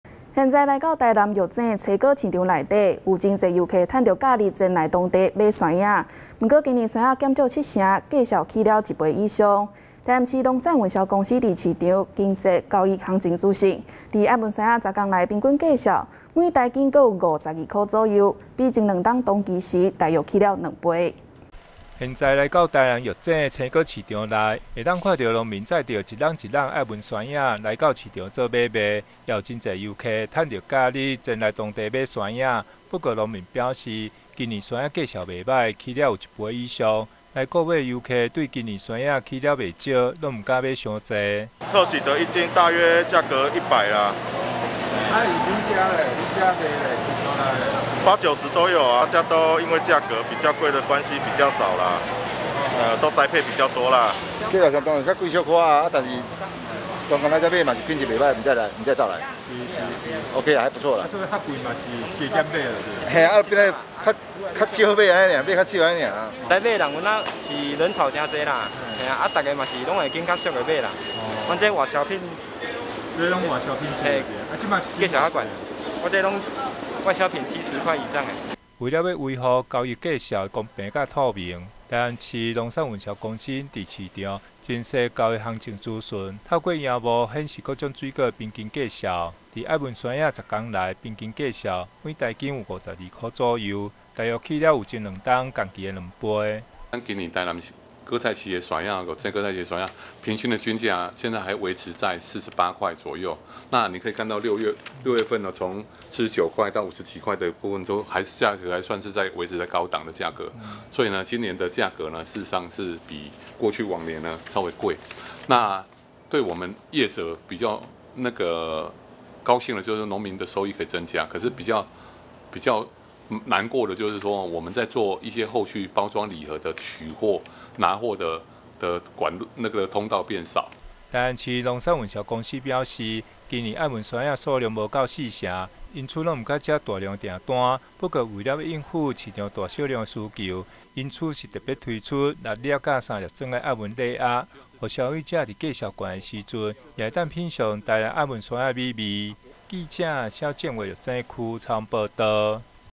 芒果銳減 玉井青果市場交易均價創新高-新永安新聞
芒果減少七成 價格漲了一倍以上 臺南市農產運銷公司在市場 增設交易行情資訊 在愛文芒果10天來的平均價格 每台斤還有52元左右 比前兩年同期時大約漲了兩倍